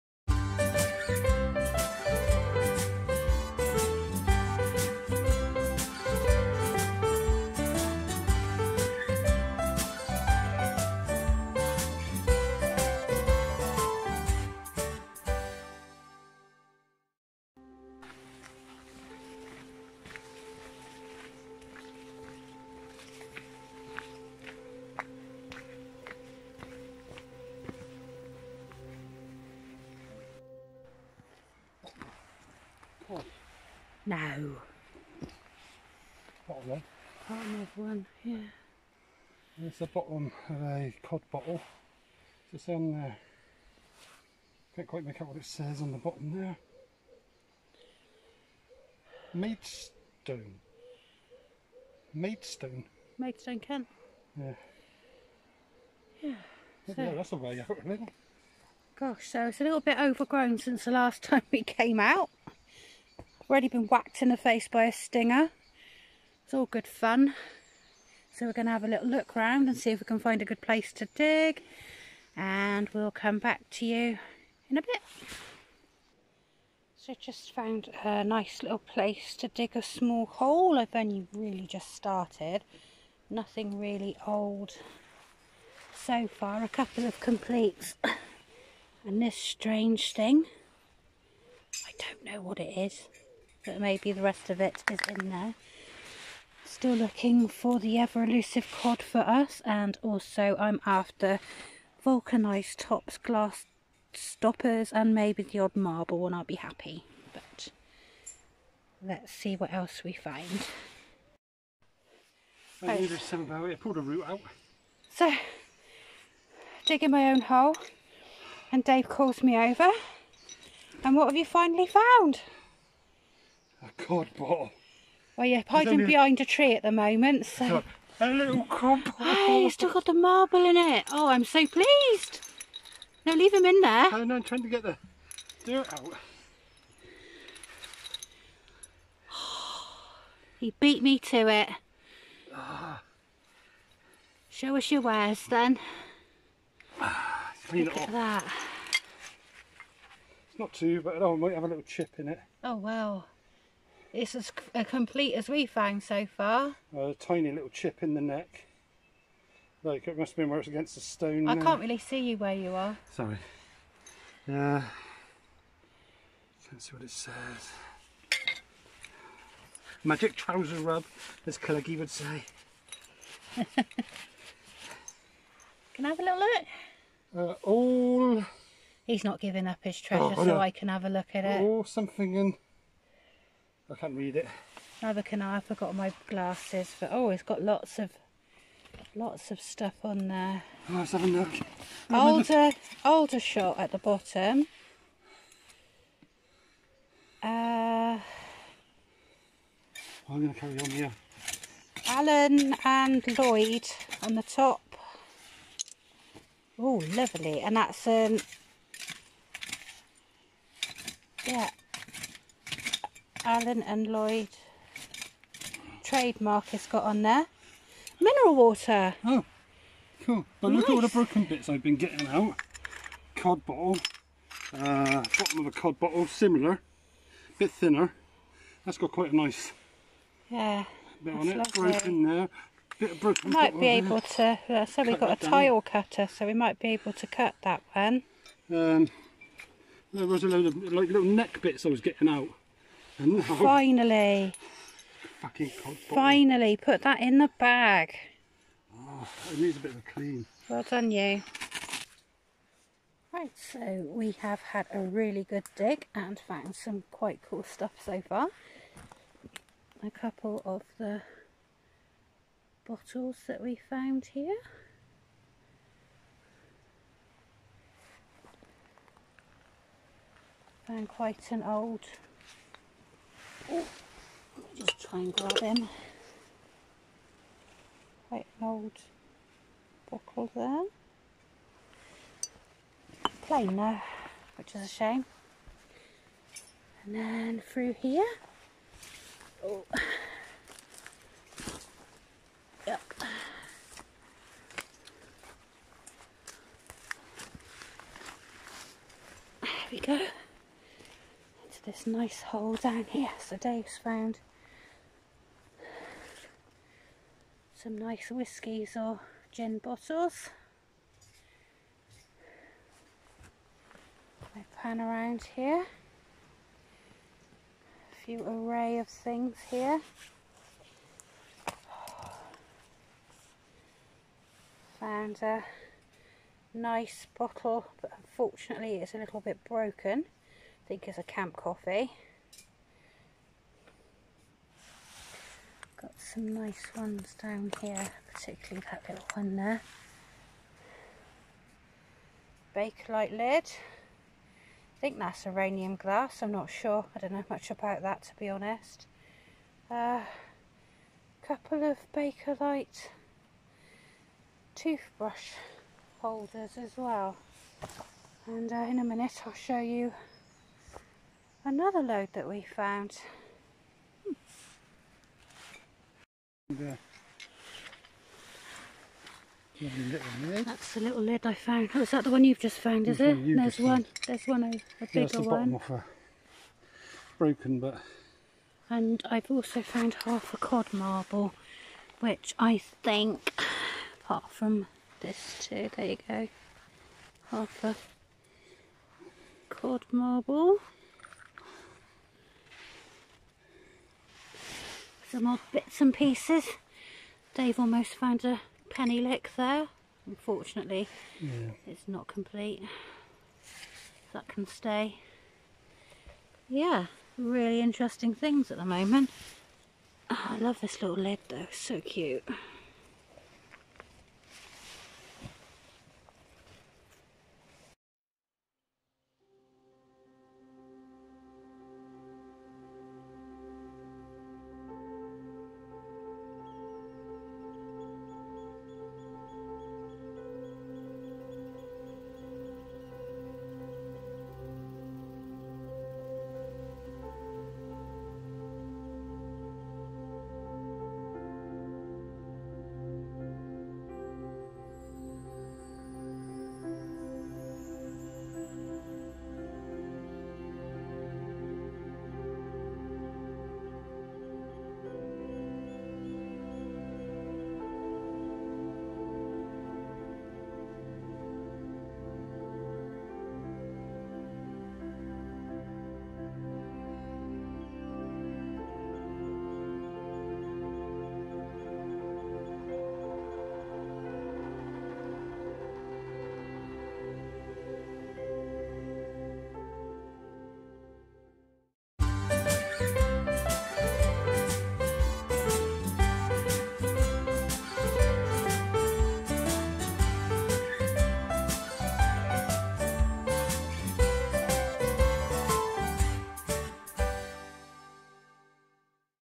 Back to the dump
mudlarking beachcombing bottledigging Mudlarking - We decided to pay a return visit to a local old dump for a spot of bottle digging. We were shocked at how overgrown it had become but that didn't stop us from our newest adventure.